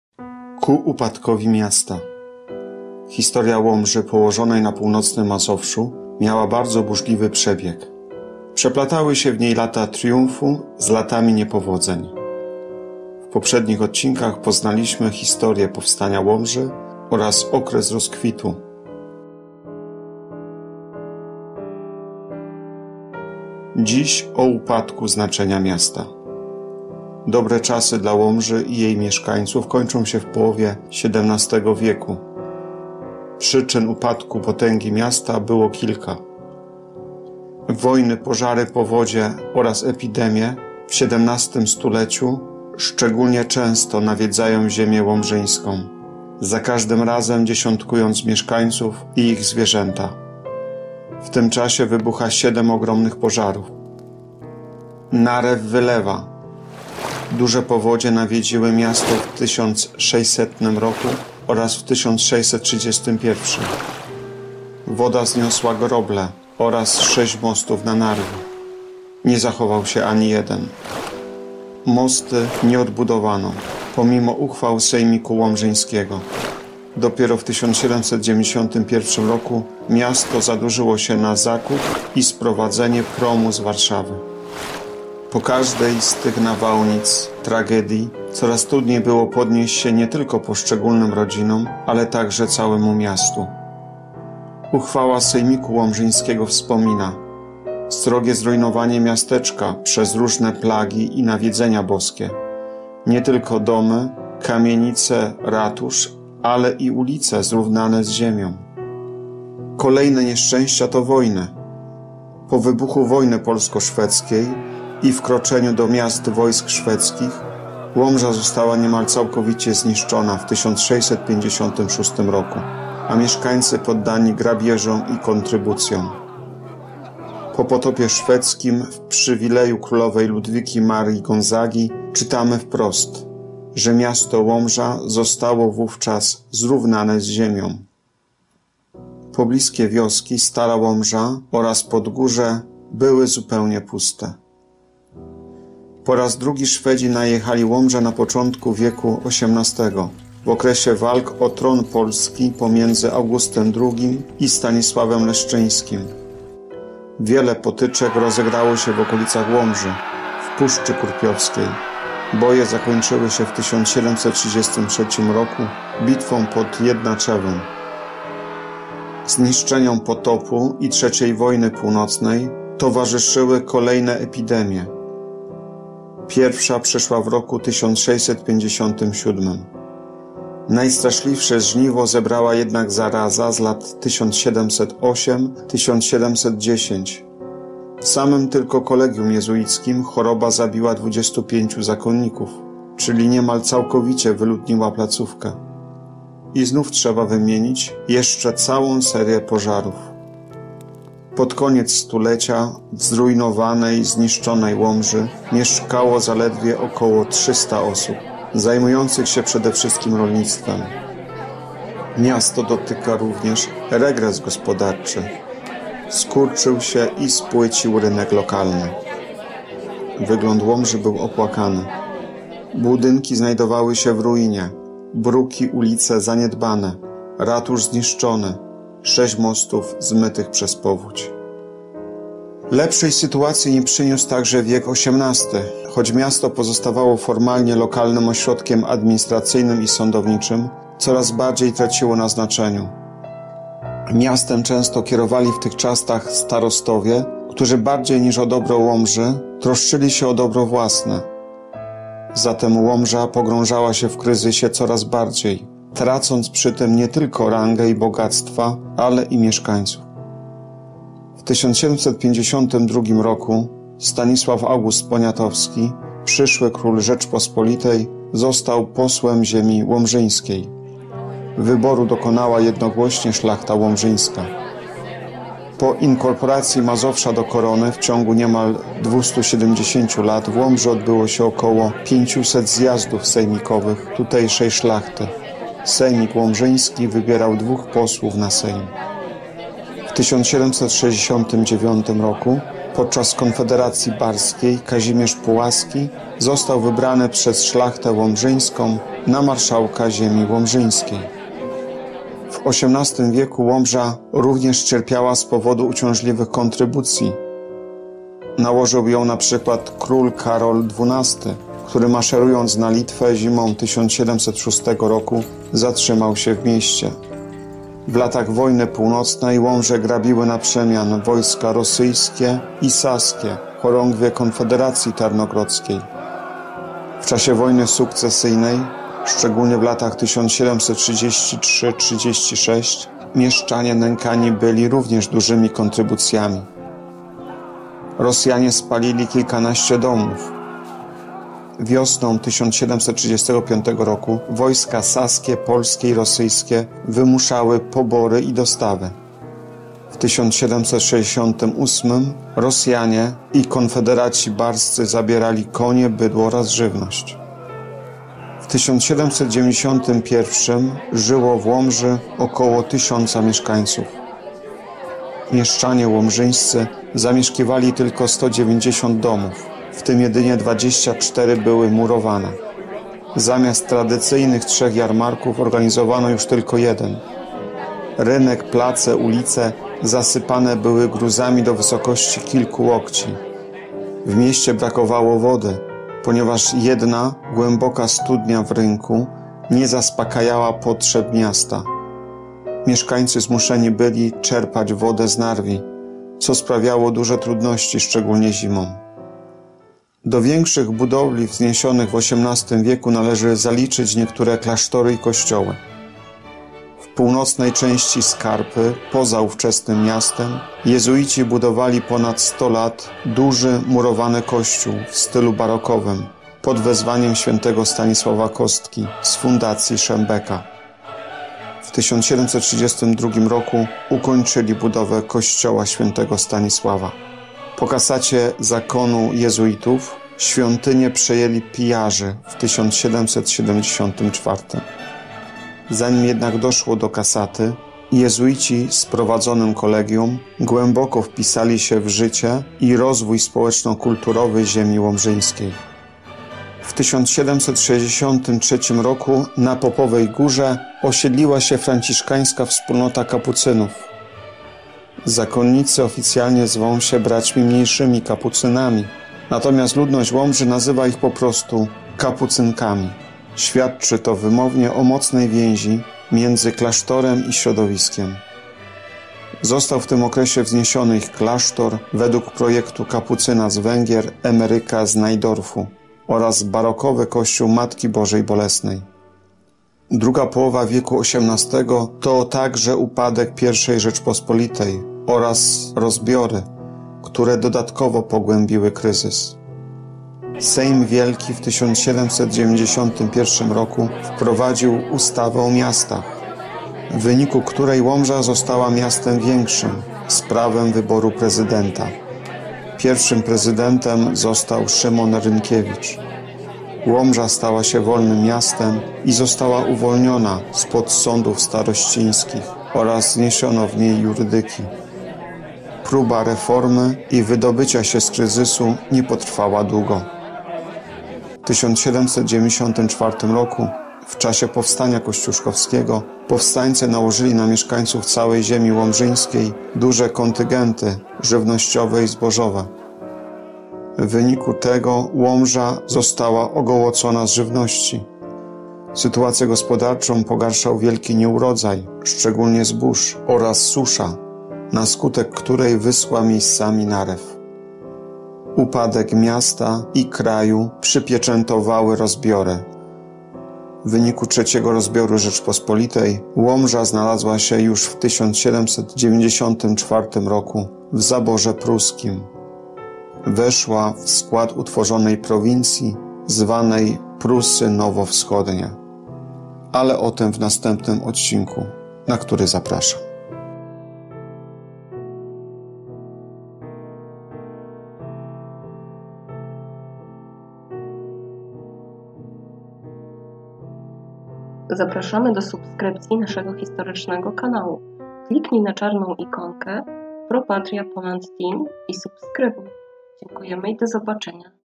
Historia Łomży. 3. Ku upadkowi miasta - XVII - XVIII wiek
Przyczyn upadku potęgi Łomży było kilka. Wojny, pożary, powodzie i zarazy, które w XVII stuleciu nawiedzają szczególnie często ziemię łomżyńską, za każdym razem dziesiątkując mieszkańców.   Druga połowa wieku XVIII to także upadek I Rzeczypospolitej i rozbiory, które dodatkowo pogłębiły kryzys.